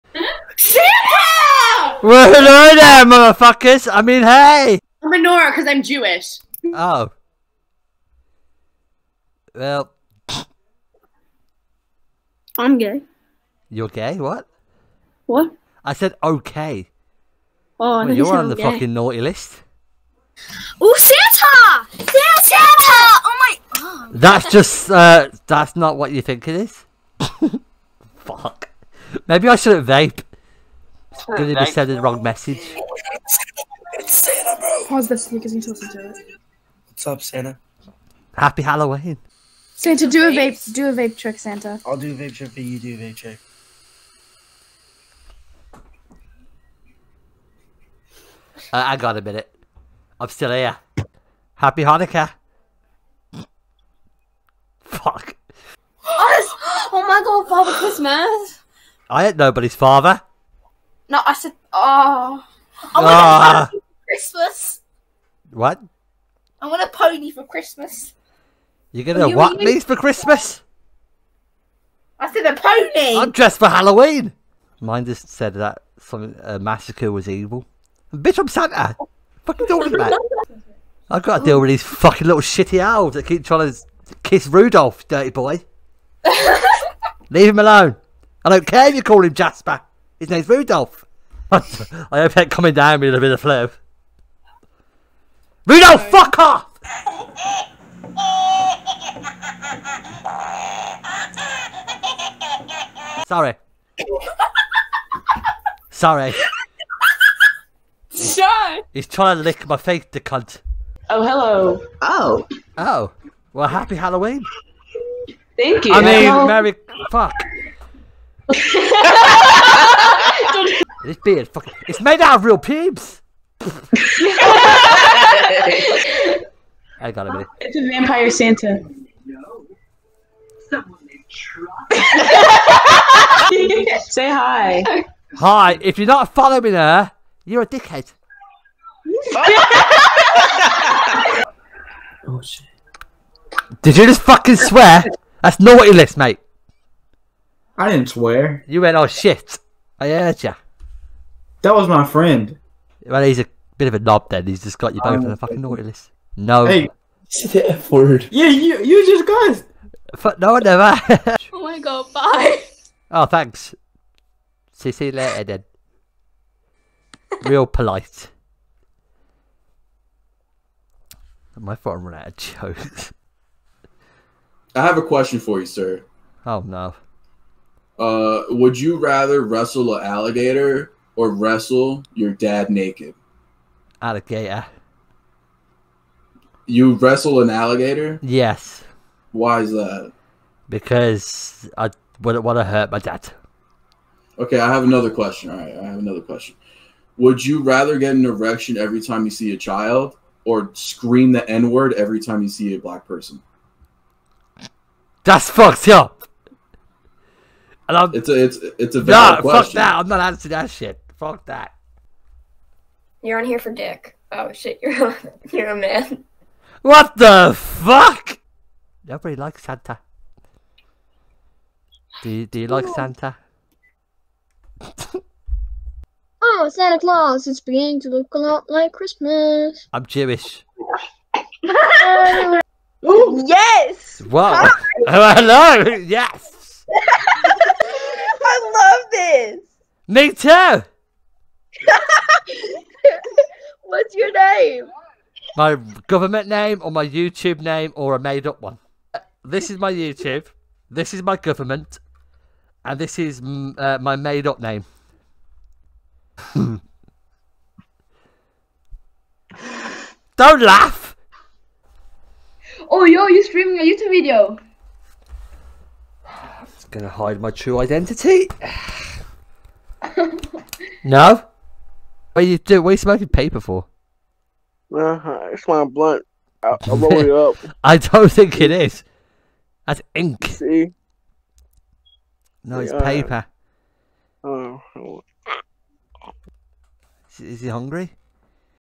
SANTA VS OMEGLE-- FUNNY MOMENTS
Hey people sorry for the lack of uploads i had a little break with my fiancee i hope you all are doing great! let me know what you think and if you want to be in the next one!